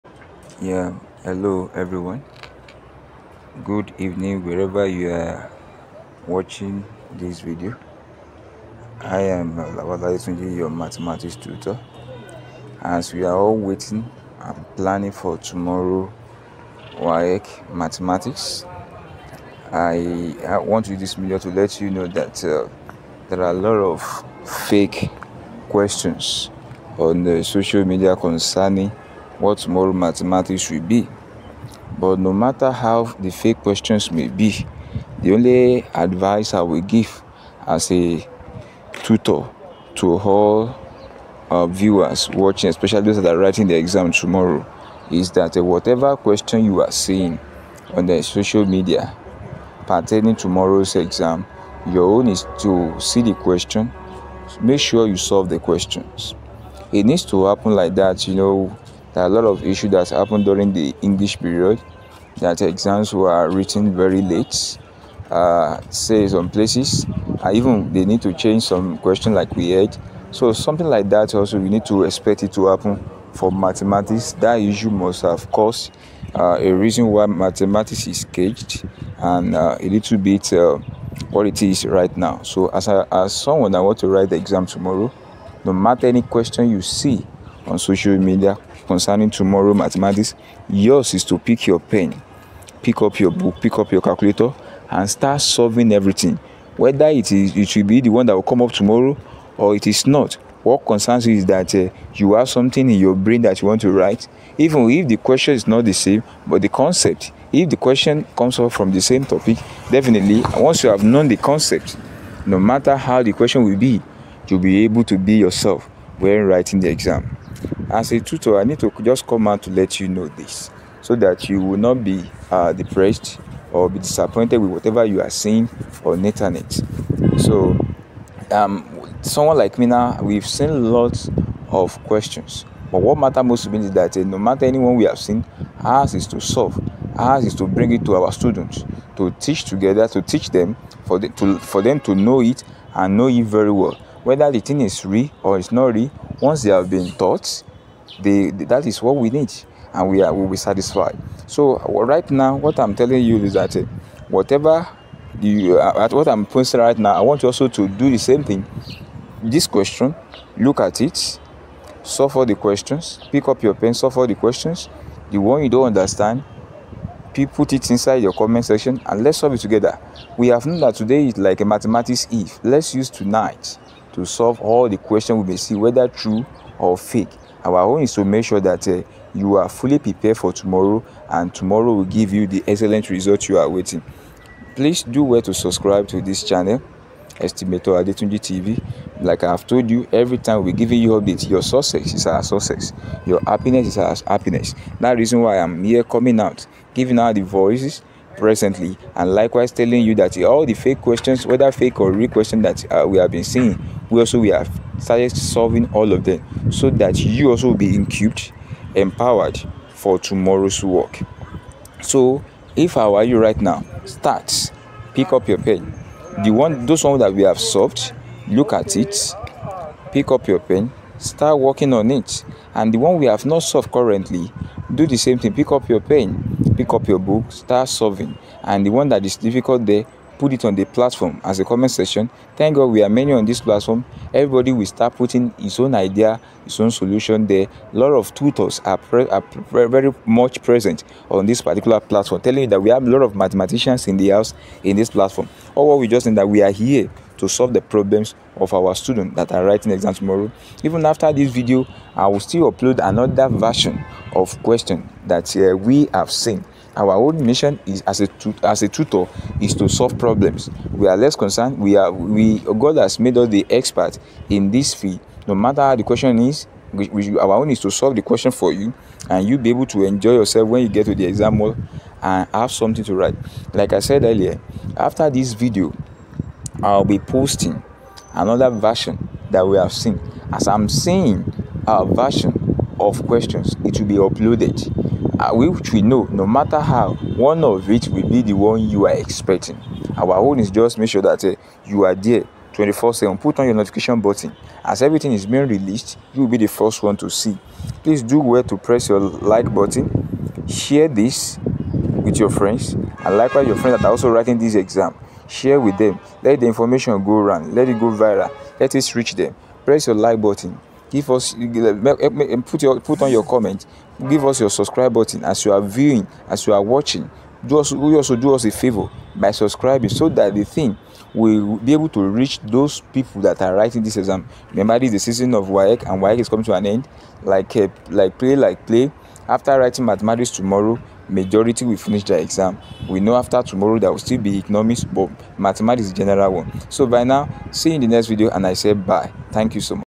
NECO 2025 Mathematics Questions update
This is video will guide you and get you prepared for A1 in NECO 2025 Mathematics. exam education @estimatoradetunji